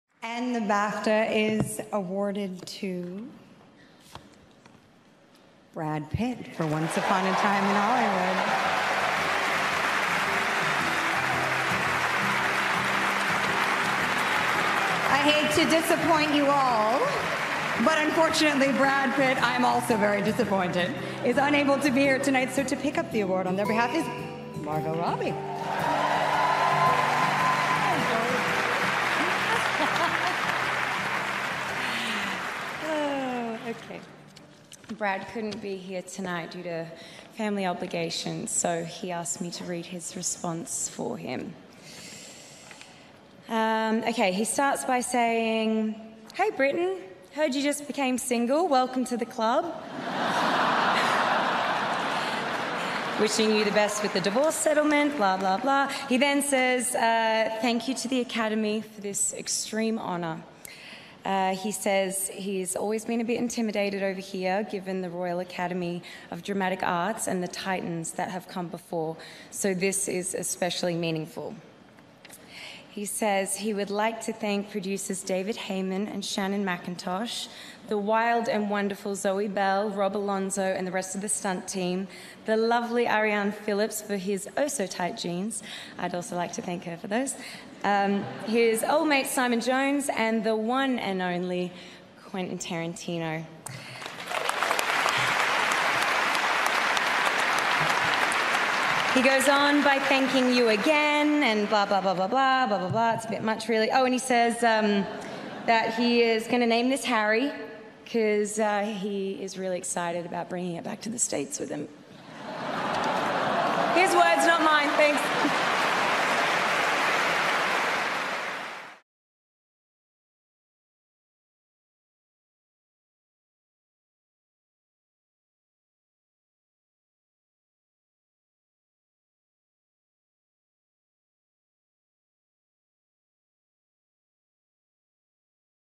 Margot Robbie's Hilarious Speech for Brad Pitt's Supporting Actor Win | EE BAFTA Film Awards 2020
Margot Robbie accepts the award for Supporting Actor on behalf of Brad Pitt at the EE BAFTA Film Awards 2020.